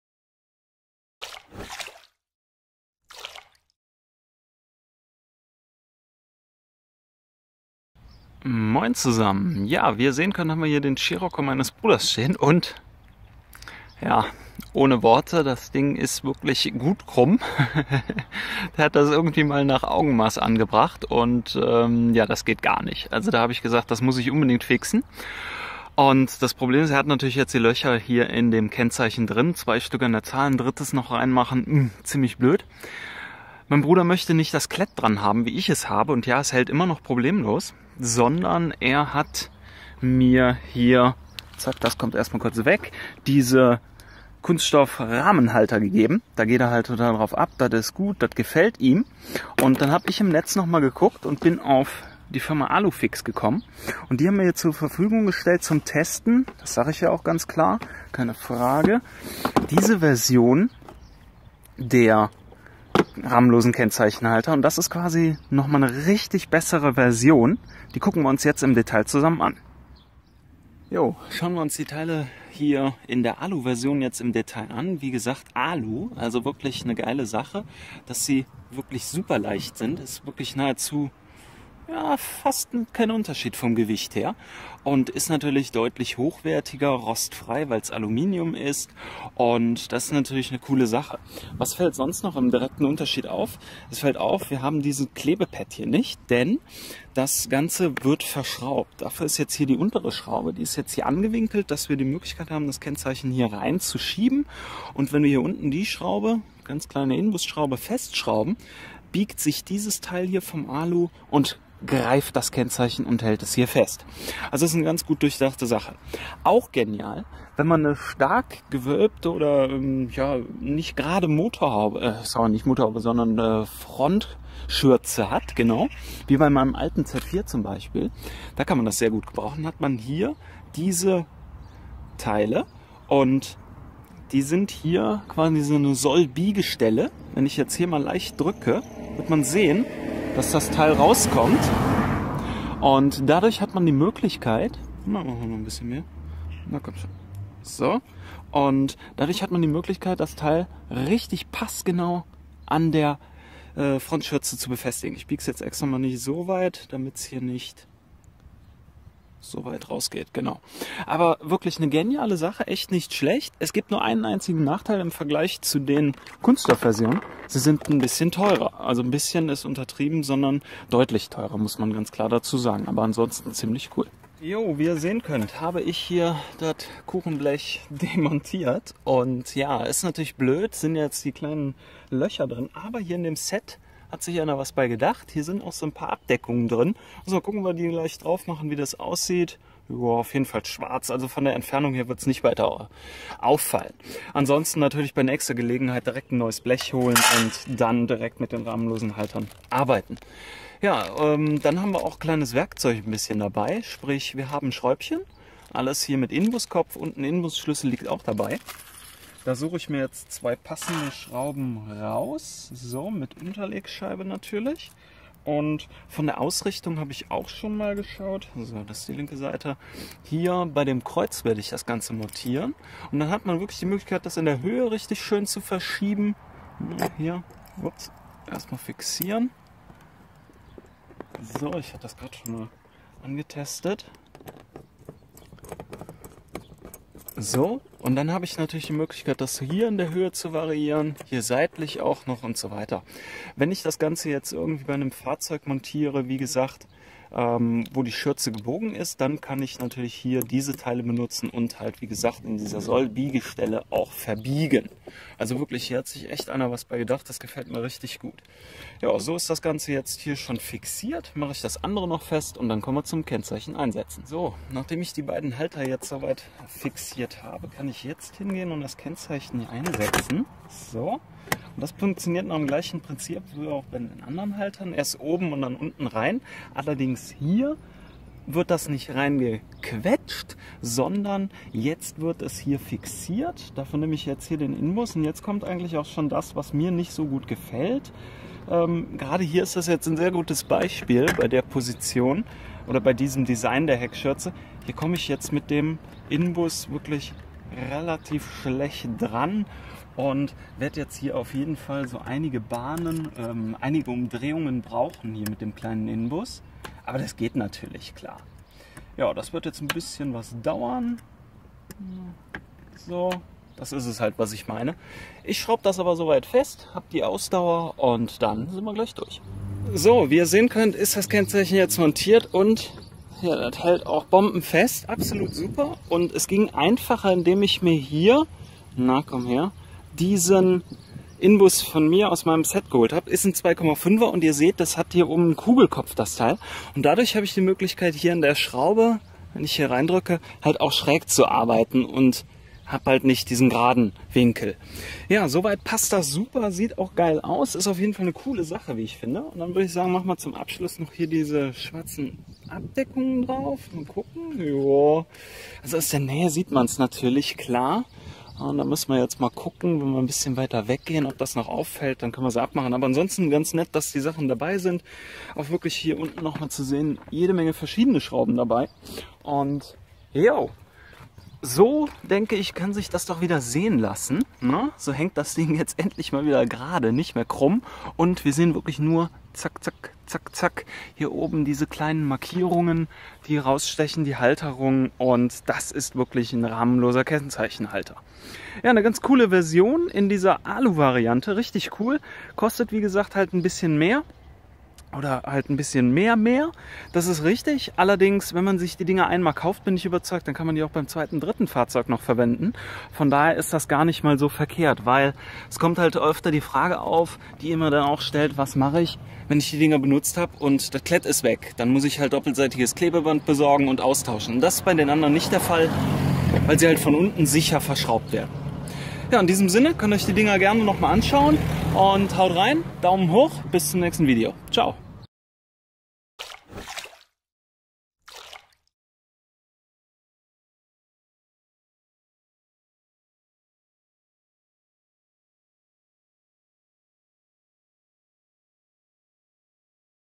Rahmenlose Kennzeichenhalter Alu - Nummernschildhalter schwarz matt eloxiert Aluminium Medes AluFixx
In diesem Video zeige ich euch die rahmenlosen Kennzeichenhalter AluFixx von medes. Diese Nummernschildhalter sind gefertigt aus Aluminium und schwarz matt eloxiert, so fallen sie fast gar nicht auf.
Weil sie aus Alu sind, ist ihre Haltbarkeit sehr lang, zudem kann Aluminium nicht rosten.